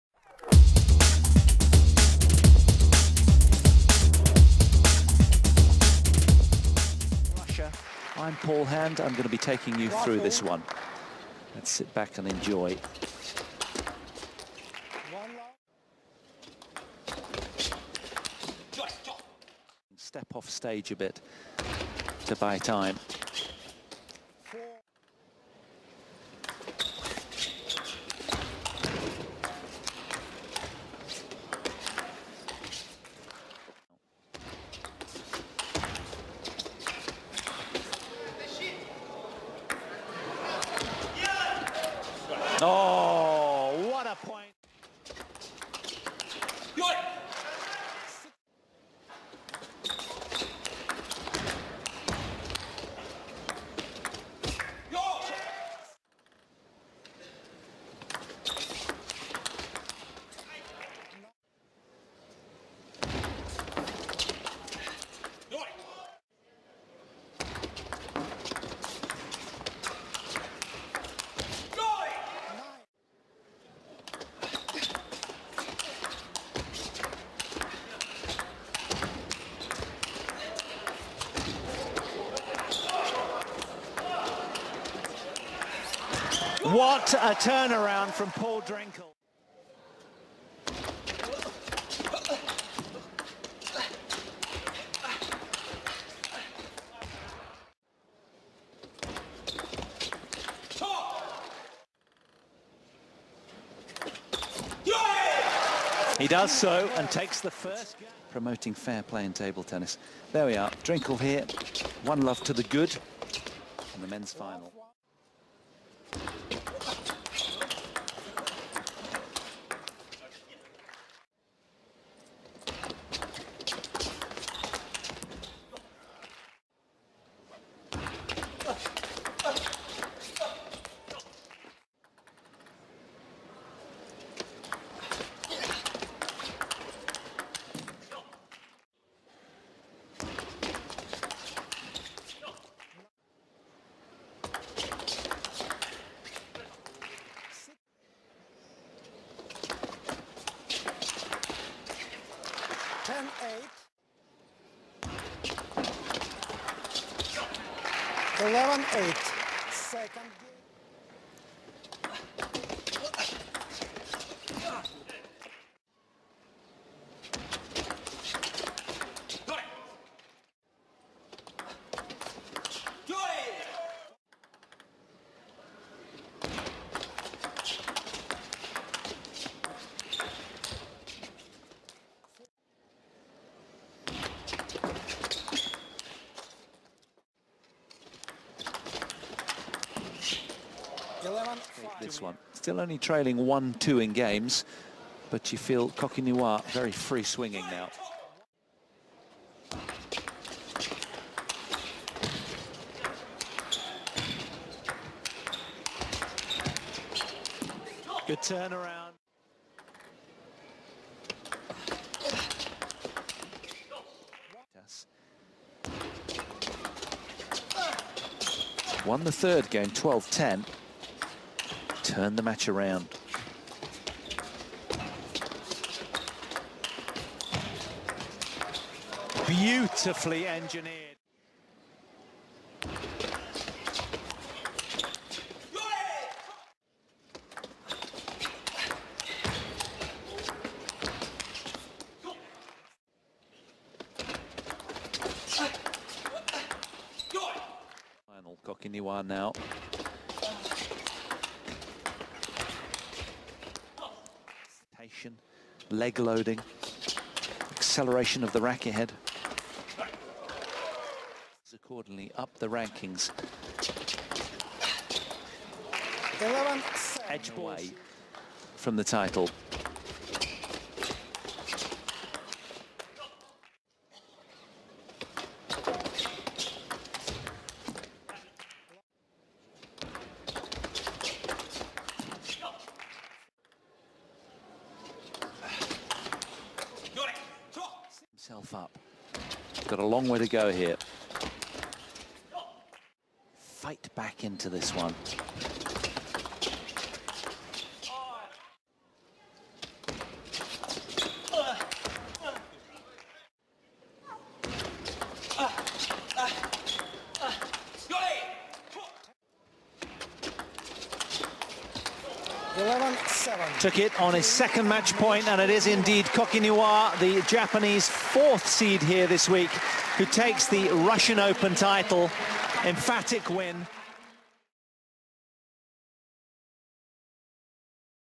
Russian Open 2014 Highlights: Koki Niwa Vs Paul Drinkhall (FINAL)
Review all the highlights from the Koki Niwa Vs Paul Drinkhall Men's Singles FINAL from the ITTF Russian Open 2014